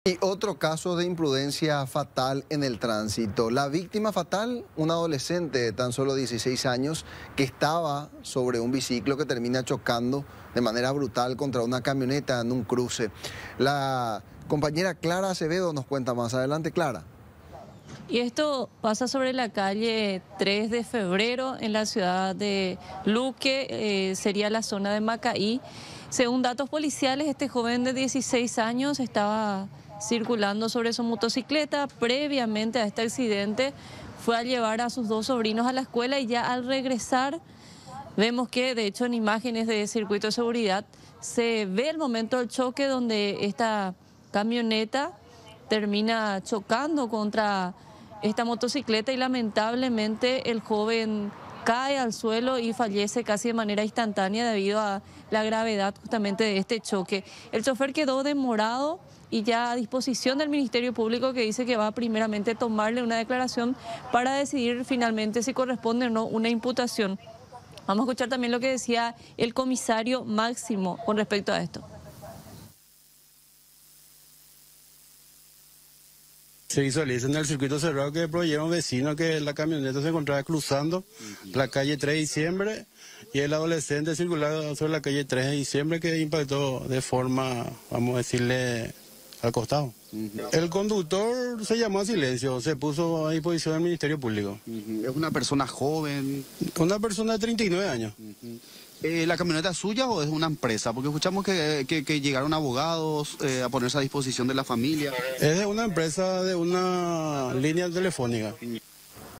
Muere menor de 16 años en accidente de tránsito
Un joven de 16 años de edad murió tras accidente de tránsito, en la ciudad de Luque. El menor iba en una motocicleta e impactó contra una camioneta.